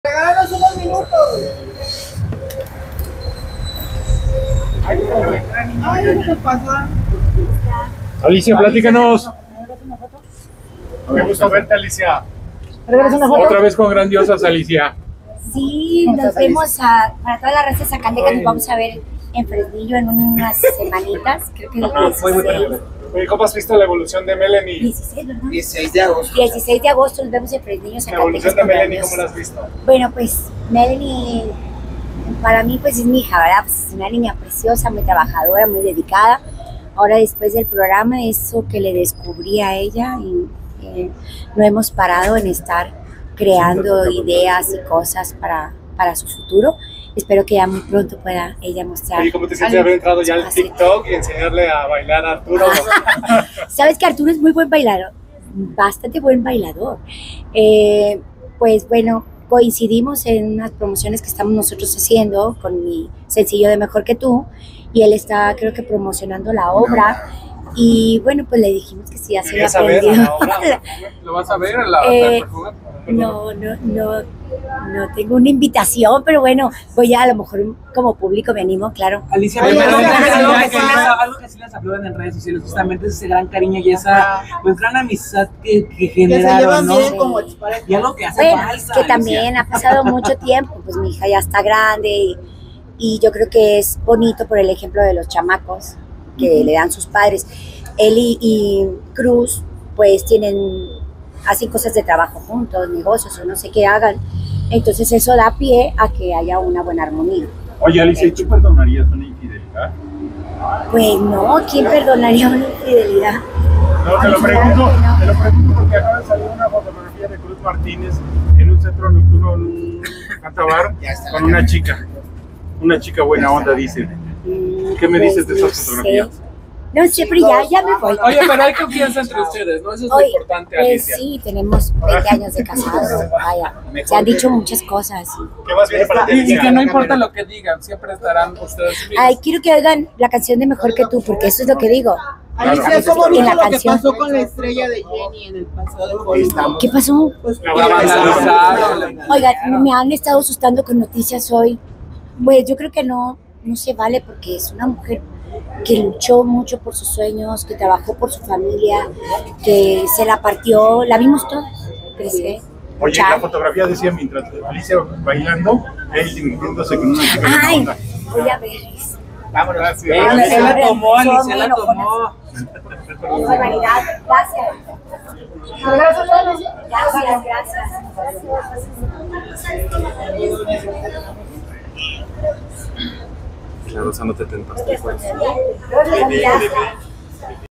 ALICIA VILLARREAL Qué dijo sobre foto de CRUZ MARTÍNEZ con chica buena onda? Irá a PERFUME?
ALICIA VILLARREAL Qué dijo sobre foto de CRUZ MARTÍNEZ con chica buena onda? Irá a PERFUME DE GARDENIA a ver a ARTURO CARMONA ?
Lleva una buena relación con Arturo y nos cuenta de sus próximas presentaciones